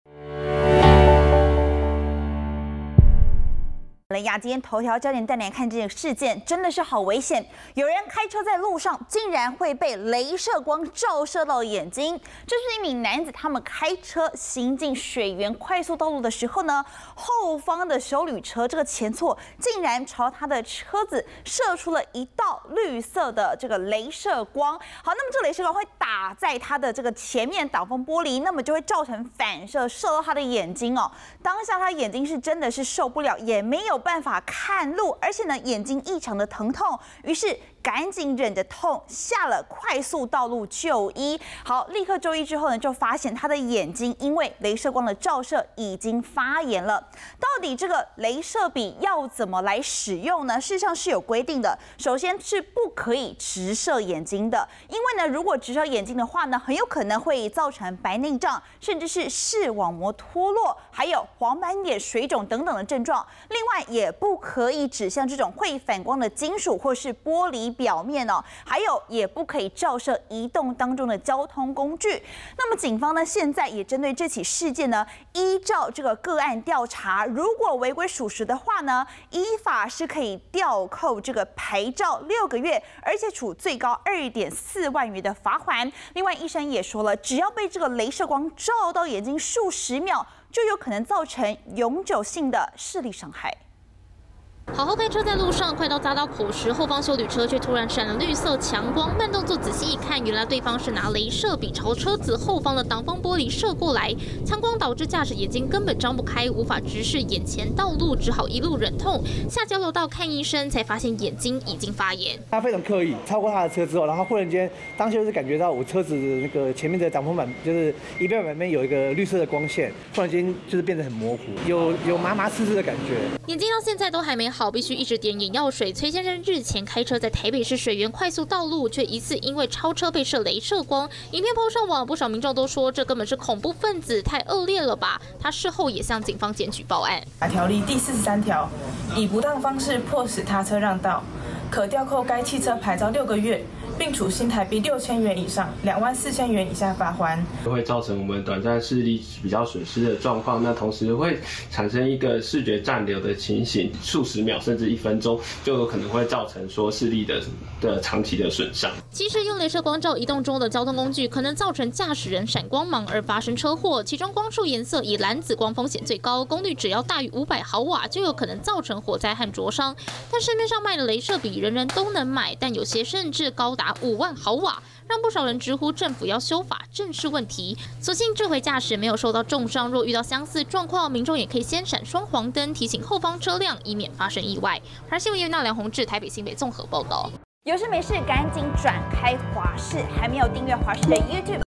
超車後突閃「雷射筆綠光」 駕駛眼睛發炎.警調查｜華視新聞 20230407
開車在路上，竟會突然被雷射光影響視線，一名崔姓男子開車行經水源快速道路時，後方休旅車前座，竟然朝他的車子射出一道綠色雷射光，造成他當下眼睛受不了，根本無法張開看路，立刻就醫後發現眼睛發炎，而眼科醫師也透露，只要被雷射光照到數10秒，就有可能造成永久性的視力傷勢，而市面上的雷射筆功率，只要大於500毫瓦，就有可能造成火災和灼傷。